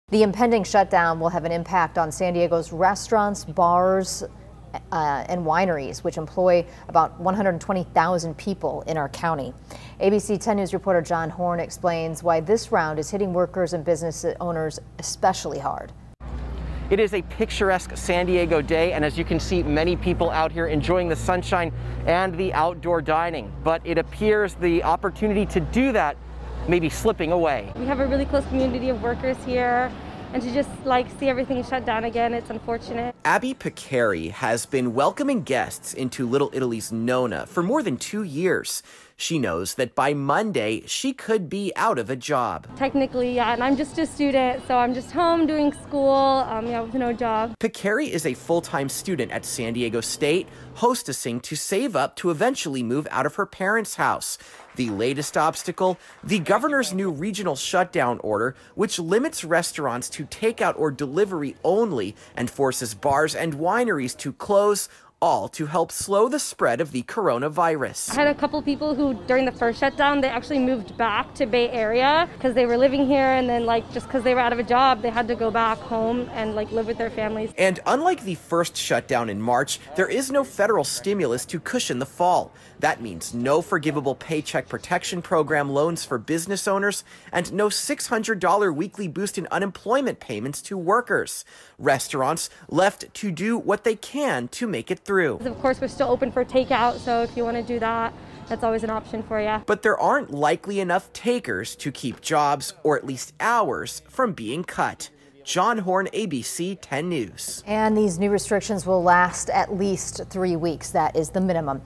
San Diego restaurants, bars brace for shutdown
Saturday was a picturesque day in San Diego with many people enjoying outdoor dining, but the opportunity is slipping away due to Governor Gavin Newsom’s new stay-at-home orders.